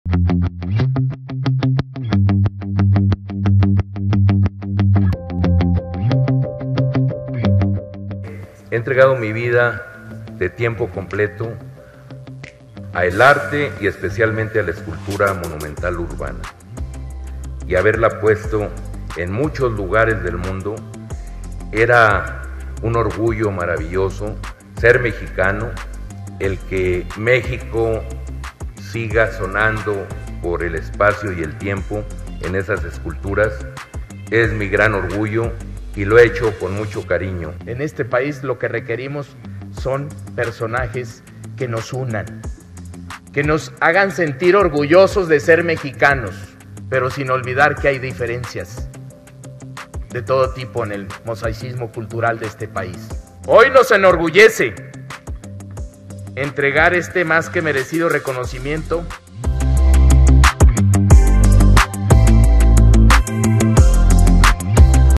“Sebastián”, autor de obra monumental
¿Conoces la escultura "El Caballito" ubicada en Paseo de la Reforma en la Ciudad de México? Esta obra es del pintor y escultor "Sebastián", mexicano que por su amplia trayectoria y aportación artística fue reconocido en el Senado.